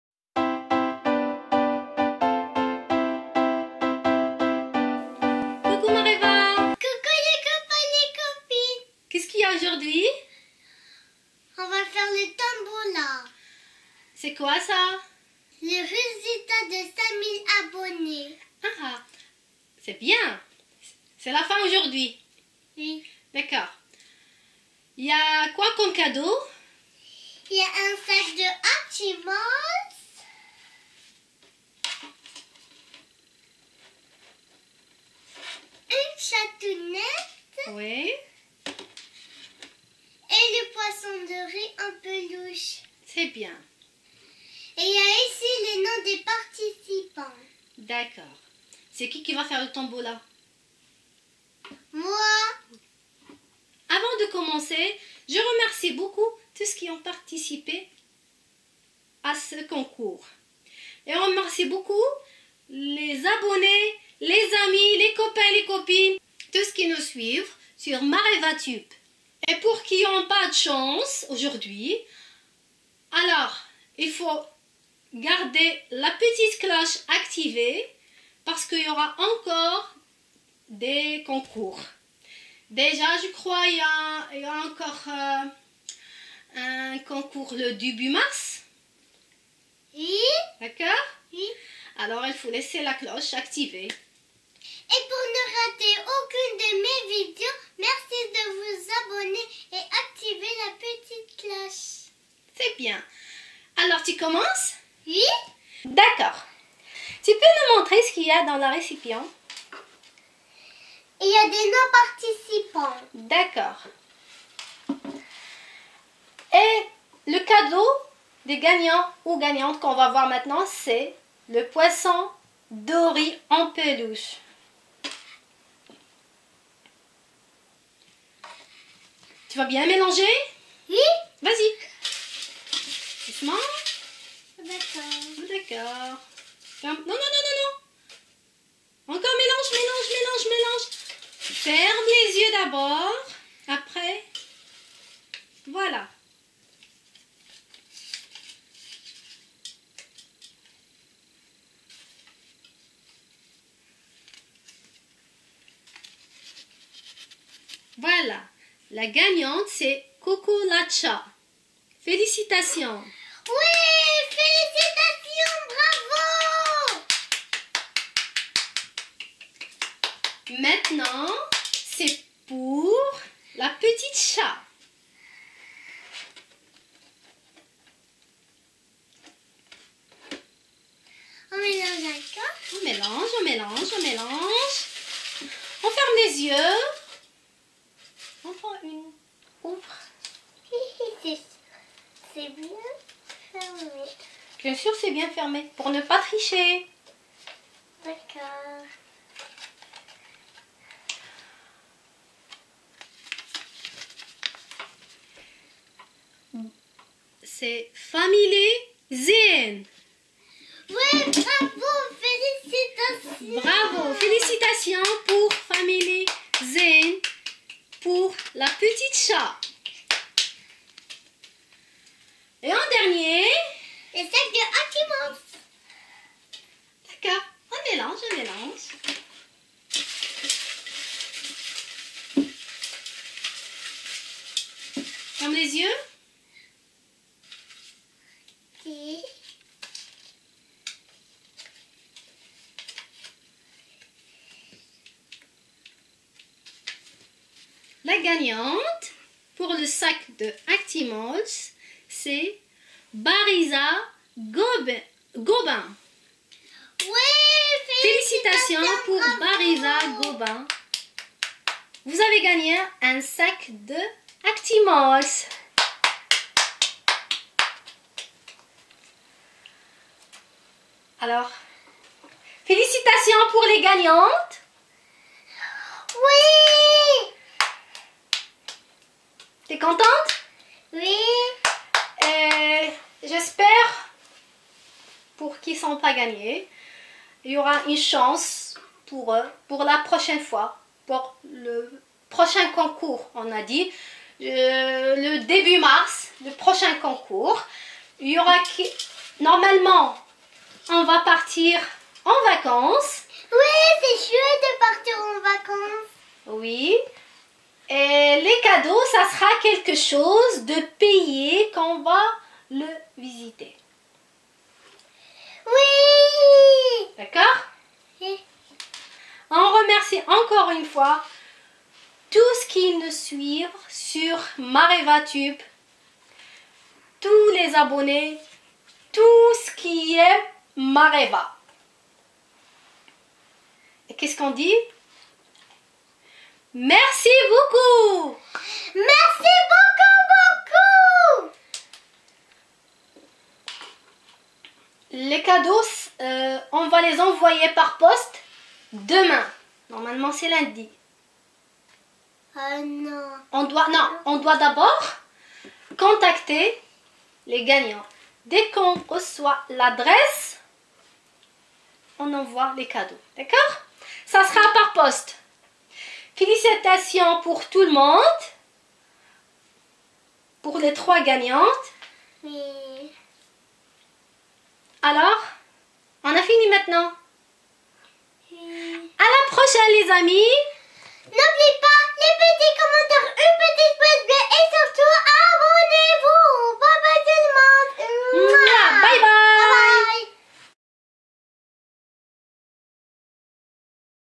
RESULTATS CONCOURS 5000 ABONNÉS-poisson Dory peluche.- palace pets -sac hatchimals-kids-toys
RESULTATS CONCOURS 5000 ABONNÉS.
mareva tiré au sort les 3 heureux gagnants qui recevront leurs cadeaux
-poisson Dory peluche.
- Whisker Haven . palace pets.
- sac hatchimals.
A très bientôt pour le prochain concours , avec encore plus de cadeaux et de gagnants
kids-toys.